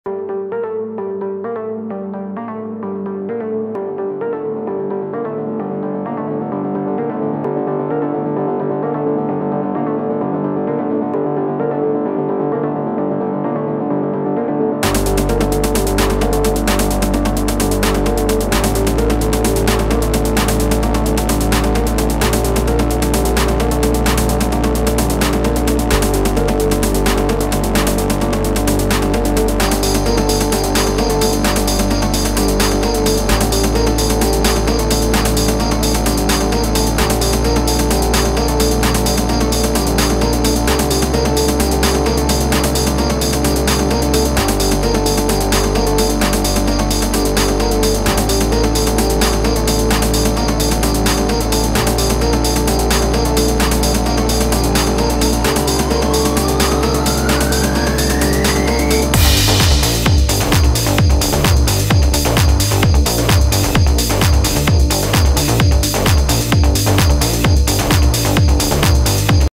sepeda anak Alfrex Arava roda tiga stenlis chrome
sepeda Alex arava roda tiga stenlis pake sandaran.

terkenal kuat dan tahan lama sudah terbukti jual ini tahunan danbrata rata buyer puas.